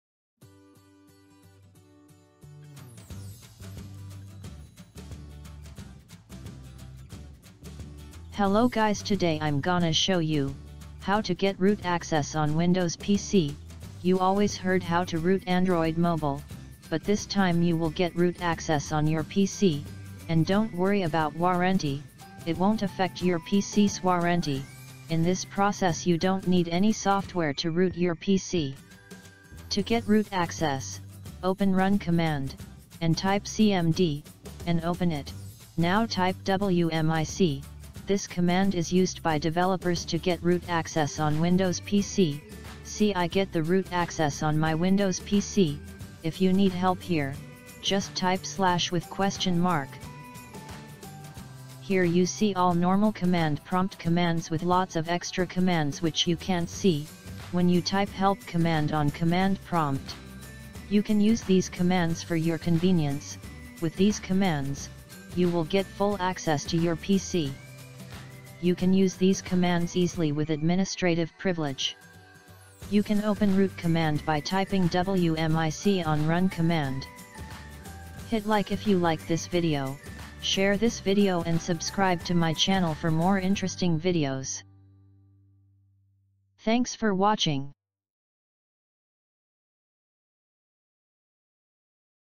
How to get Root Access on Windows PC - How To?
Always heard about how to root android mobile, but in this video you see how to get root access on Windows PC.